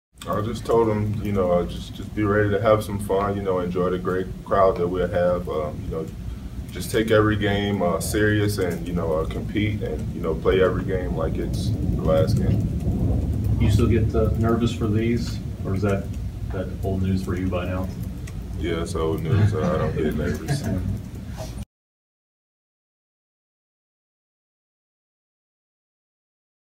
KU's Frank Mason describes what he's told the freshmen about their first game at Allen Fieldhouse
Kansas guard Frank Mason talks about advice he's given to younger teammates before Tuesday's exhibition opener against Washburn.